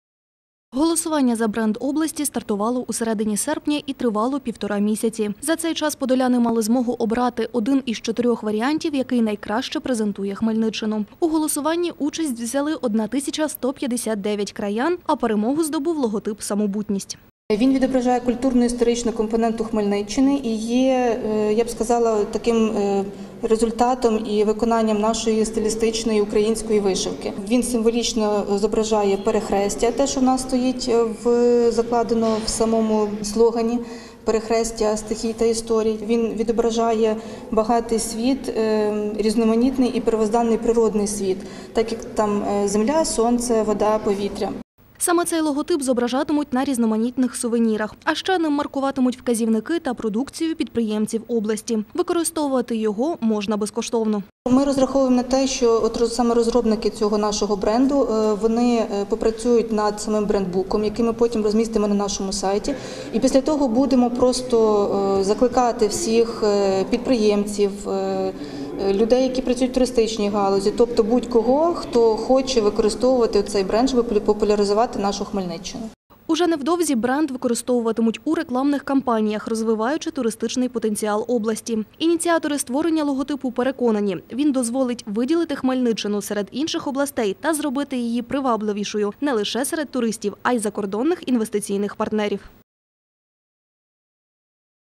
Хмельниччина відтепер брендова
Хмельниччина відтепер має власний бренд. Нагадаємо, його визначали протягом двох місяців жителі області шляхом Інтернет-голосування.  Обирали із чотирьох варіантів, розроблених київськими фахівцями.